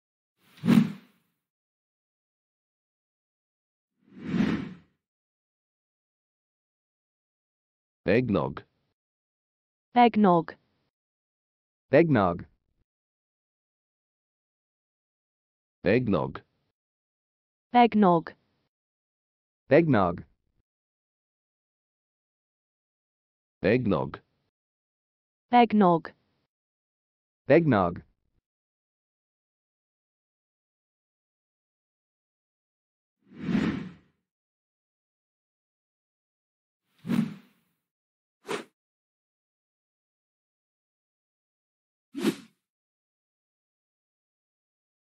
How to pronounce "Eggnog"?
Learn how to pronounce "Eggnog" in English, improve your accent and sound like a native speaker!

"Eggnog, egg nog or egg-nog, historically also known (when alcoholic beverages are added) as milk punch or egg milk punch, is a rich, chilled, sweetened, dairy-based beverage. It is traditionally made with milk, cream, sugar, whipped egg whites, and egg yolks (which gives it a frothy texture, and its name). In some contexts, distilled spirits such as brandy, rum, whisky or bourbon are added to the drink.
Throughout Canada and the United States, eggnog is traditionally consumed over the Christmas season, from late October until the end of the holiday season. A variety called Ponche Crema has been made and consumed in Venezuela and Trinidad since the 1900s, also as part of the Christmas season. During that time, commercially prepared eggnog is sold in grocery stores in these countries.
Eggnog is also homemade using milk, eggs, sugar, and flavorings, and served with cinnamon or nutmeg. While eggnog is often served chilled, in some cases it is warmed, particularly on cold days (similar to the way mulled wine is served warm). Eggnog or eggnog flavoring may also be used in other drinks, such as coffee (e.g. an "eggnog latte" espresso drink) and tea, or to dessert foods such as egg-custard puddings."
(source: Wikipedia)